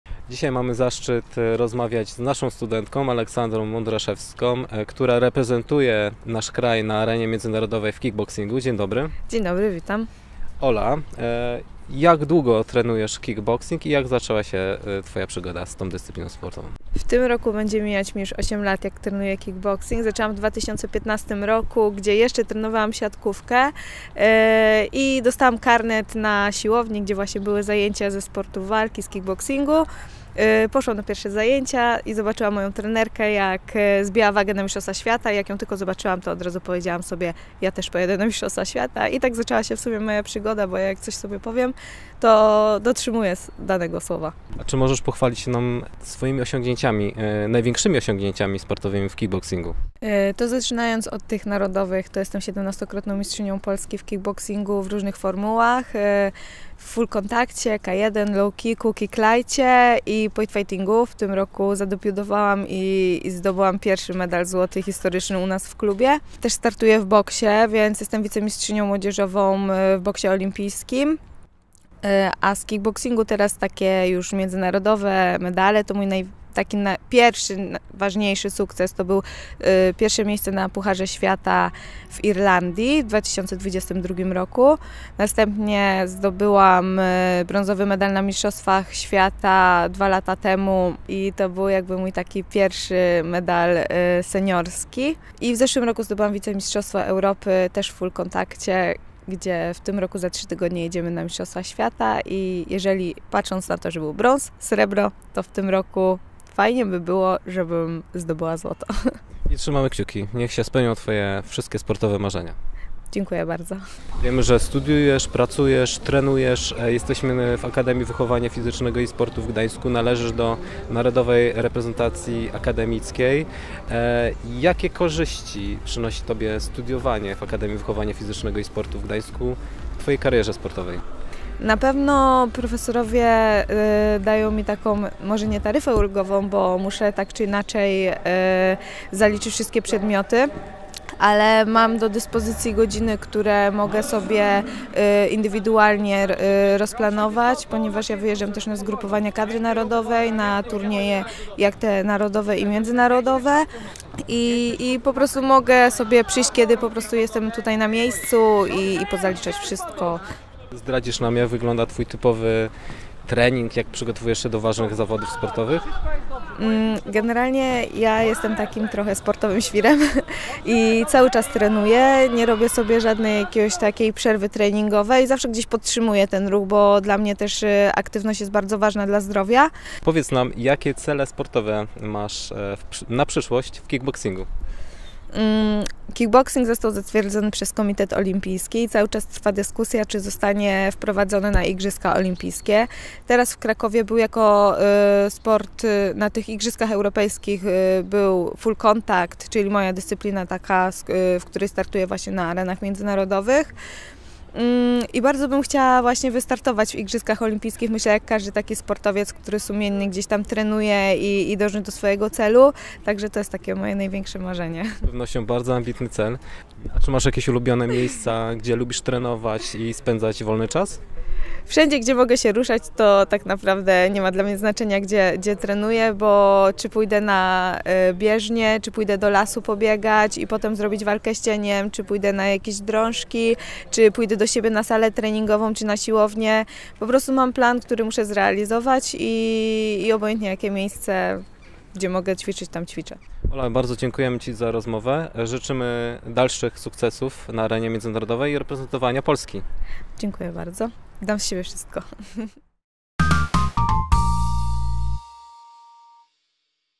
Wywiad z Aleksandrą Mądraszewską | AWFiS 2023
Zachęcamy do obejrzenia wywiadu z naszą studentką ‼️🎥

Aleksandra Mądraszewska studiuje w Akademii Wychowania Fizycznego i Sportu w Gdańsku na kierunku Wychowanie Fizyczne i należy do Narodowej Reprezentacji Akademickiej. Na co dzień trenuje w Gdańskim Klubie Sportowym „Champion”  i reprezentuje nasz kraj na arenie międzynarodowej w kickboxingu, w formule Full Contact. 🥊

Rozmawia: Mikołaj Polak
Operator kamery: Beata Zarach
Biuro Promocji AWFiS w Gdańsku